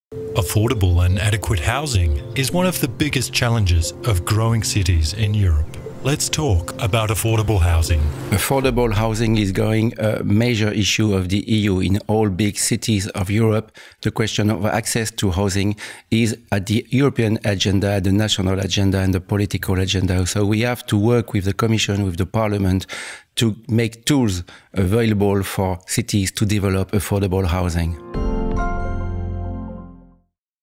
Let's Talk About Affordable Housing! Laurent Ghekiere
Housing for All. Affordable Housing in Growing Cities in Europe
4-5 December 2018
Wiener Wohnen, Rosa-Fischer-Gasse 2
1030 Vienna

Which legal and financial conditions are necessary in Europe to increase investment in affordable housing?
Which benefits does affordable housing provide for European cities and the economy?

Mayors from different European cities, local politicians, housing experts, housing providers, tenants’ representatives and scientists will discuss these questions with the EU legislators comprehensively.
The findings of the EU Urban Agenda – Housing Partnership will be presented. The City of Vienna will offer study visits to present the Viennese model of social housing.